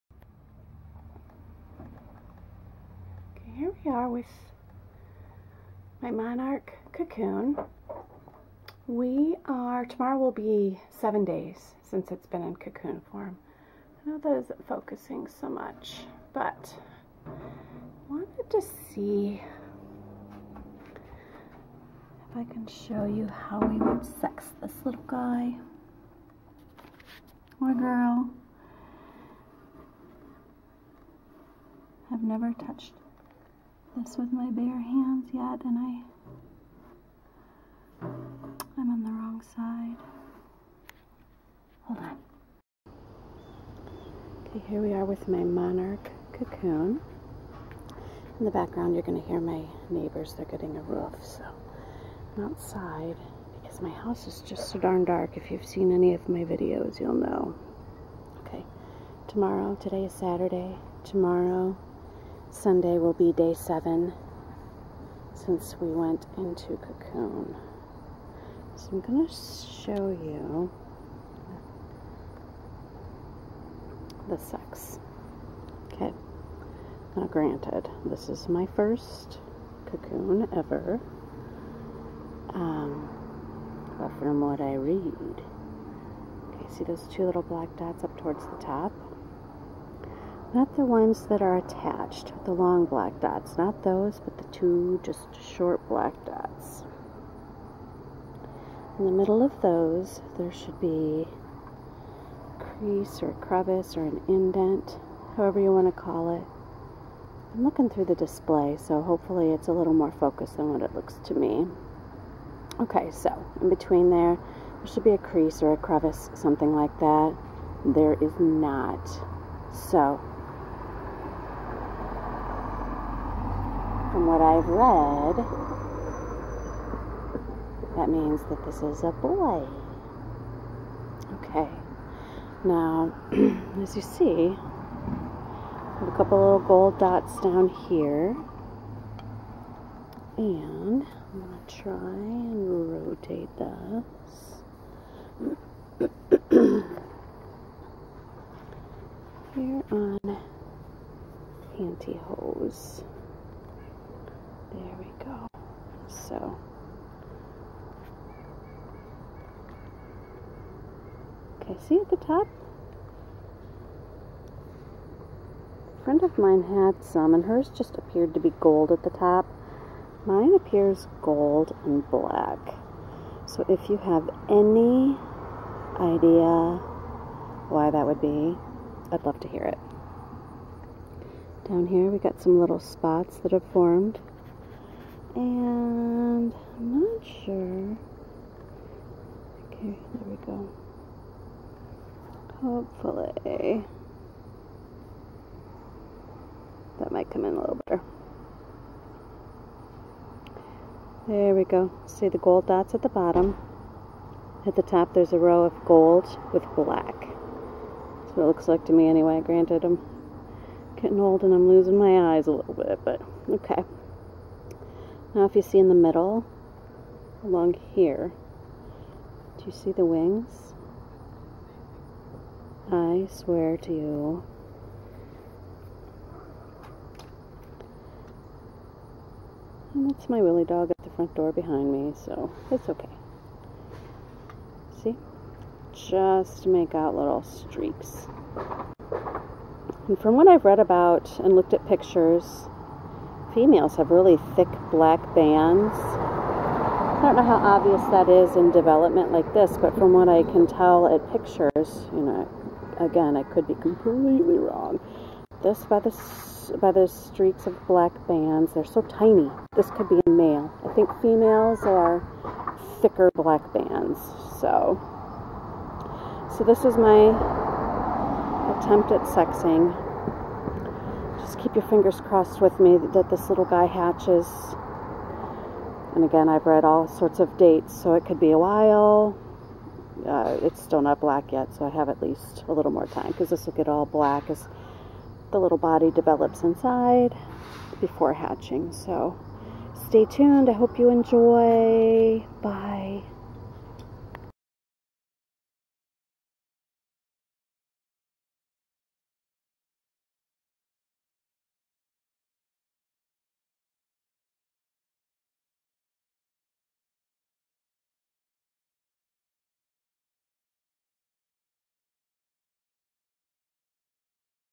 Sexing Monarch butterfly cocoon chrysalis, pupa
Sexing my cocoon. All along I've referred to it, as him. From what I can tell, I might be right!!